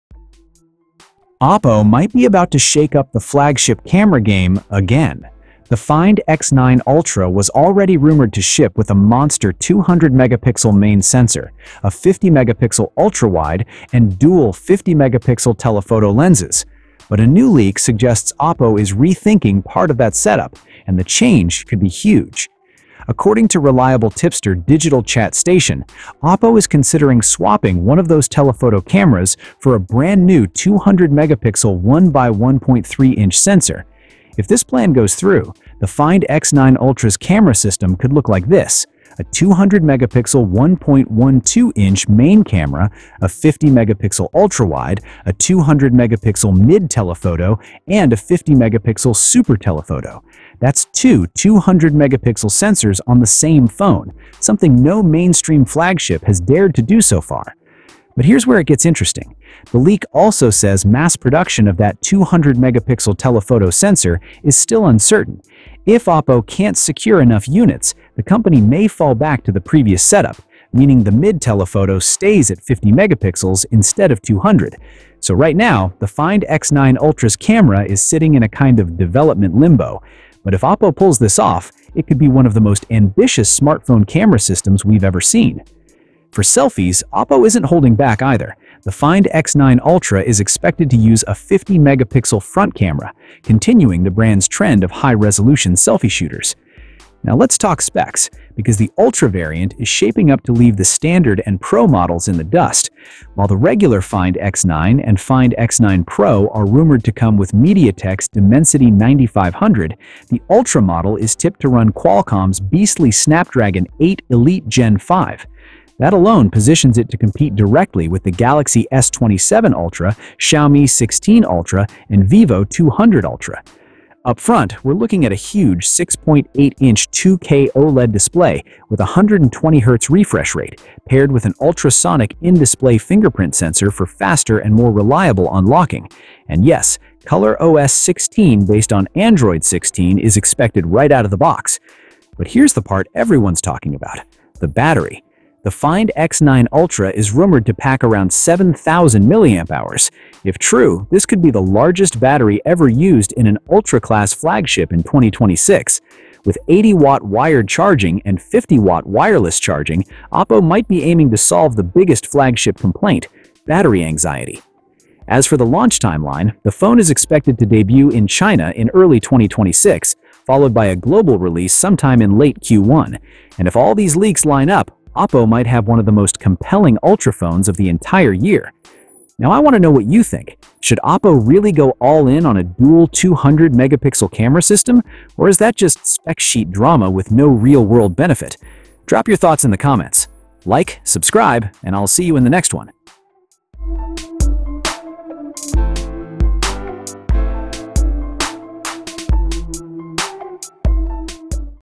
Oppo Find X9 Ultra Could Get Dual 200MP Cameras — Huge Leak!
The Oppo Find X9 Ultra is shaping up to be one of the most powerful flagship smartphones of 2026. New leaks reveal that Oppo may upgrade the camera setup with a massive 200MP mid-telephoto sensor, joining the already rumored 200MP 1/1.12” main camera, 50MP ultrawide, and 50MP super-telephoto lens. This could make the Find X9 Ultra the first flagship with dual 200MP cameras. If production challenges arise, Oppo may revert to its earlier dual-50MP telephoto configuration.

The Find X9 Ultra is also expected to feature a 6.8-inch 2K 120Hz OLED display, Snapdragon 8 Elite Gen 5 processor, 7,000mAh battery, 80W fast charging, 50W wireless charging, and ColorOS 16 based on Android 16. With this hardware, Oppo aims to compete head-on with the Samsung Galaxy S27 Ultra, Xiaomi 16 Ultra, and Vivo X200 Ultra.

The smartphone is rumored to launch in early 2026 in China, with a global launch expected in late Q1 2026.
This video covers all the details, leaks, specs, camera rumors, performance expectations, and early comparisons.

📌 Topics Covered

• Oppo Find X9 Ultra camera leaks
• 200MP telephoto rumor
• Snapdragon 8 Elite Gen 5 performance
• 6.8-inch 2K OLED 120Hz display
• 7,000mAh battery capacity and charging
• ColorOS 16 / Android 16 features
• Oppo Find X9 Ultra launch date
• Comparison with 2026 flagship phones